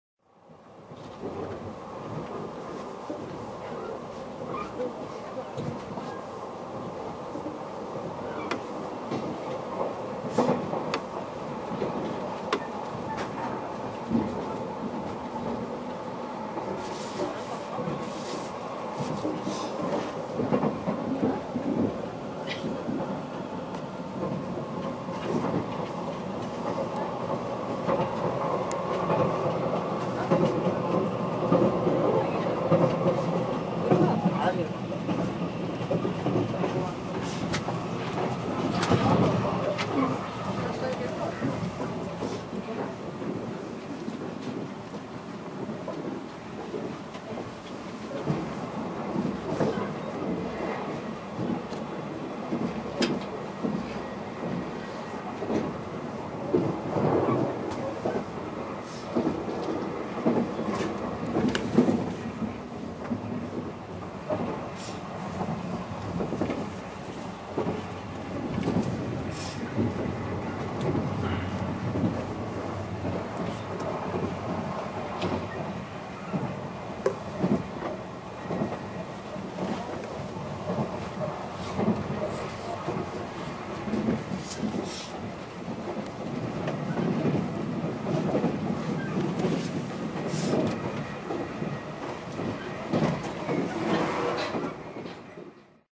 Akebono train
Sleeper train Akebono. Travel from Akita to Ueno (March 30-31, 2012).